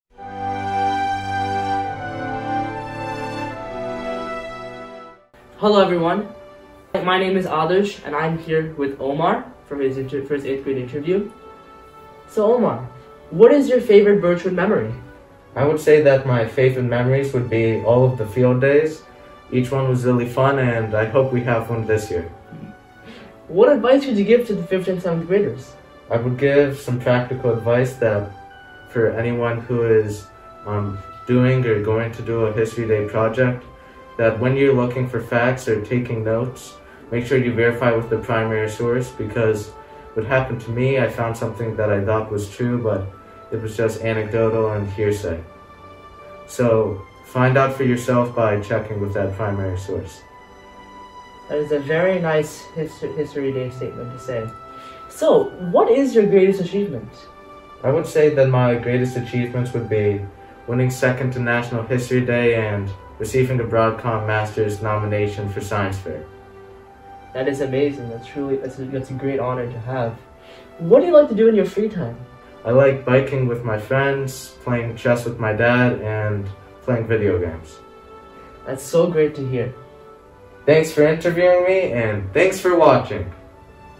Omar
Omar, an 8th-grade student at Birchwood School of Hawken, is interviewed by classmate Adharsh about his Birchwood experience. He discusses his favorite memories, proudest moments, and his interests.